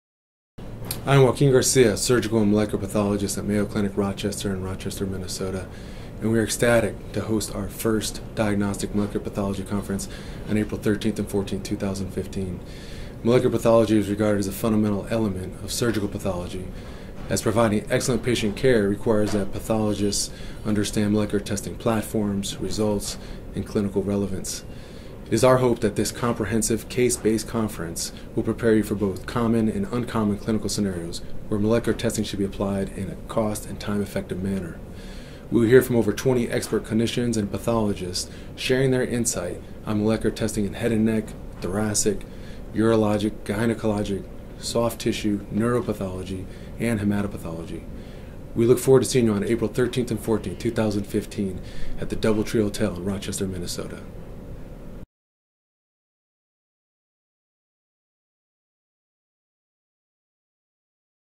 Diagnostic Molecular Pathology Conference
Dr. Joaquin Garcia, a surgical and molecular pathologist at Mayo Clinic, discusses the upcoming Diagnostic Molecular Pathology conference being held April 13-24, 2015, at the Double Tree Hotel in Rochester, MN.